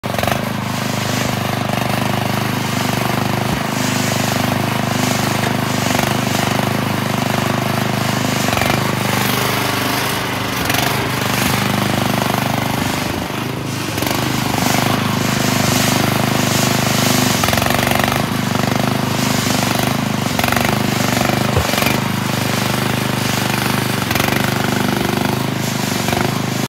2024 Miva VA08 Mini Excavator For Virtual Auction September 14th, 2024
2024 Miva VA08 Mini Excavator W/ Backfill Blade, 7IN Rubber Tracks, Auxiliary Hydraulics, 12IN Digging Bucket, Fixed Thumb Engine: 420cc Gas S/N: VA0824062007 Hours: 0

September 14th, 2024 Public Virtual Online Auction. Auction Starts at 8:00 A.M
No Deposit Required to Register & Bid Online, Online Bidding is open now. NO RESERVES!
Preview is Monday through Friday from 7:00 AM to 3:30 PM